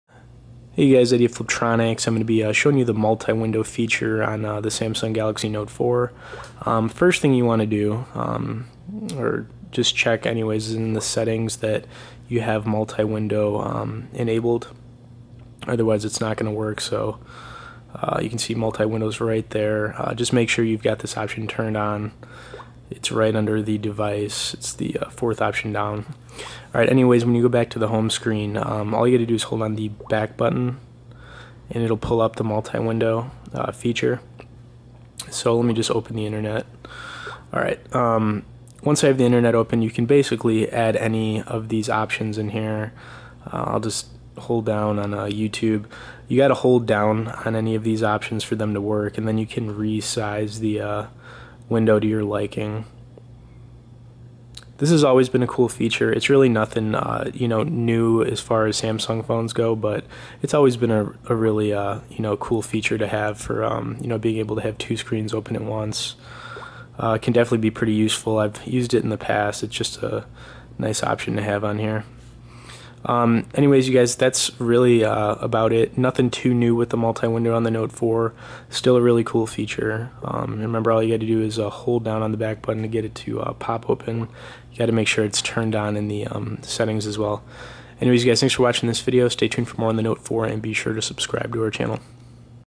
Samsung Galaxy Note 4: Multitasking with Multi Window Mode - Fliptroniks.com
Best Note 4 Case
Best Note 4 Battery
Best Note 4 Charger

Using multi window on the samsung galaxy note 4 is a useful feature for opening 2 screens at once to get more done. I myself have used this feature in the past on previous galaxy devices and found it to be quite useful. To use this feature go into the setting on your galaxy note 4 and scroll down to multi window and slide it to the right to activate it. Next go back to your home screen and hold down on the back button until the multi window option appears. Once its open hold and drag any 2 applications into the middle of the screen. This will enable you to have lots of different options open on your samsung galaxy note 4 simultaneously.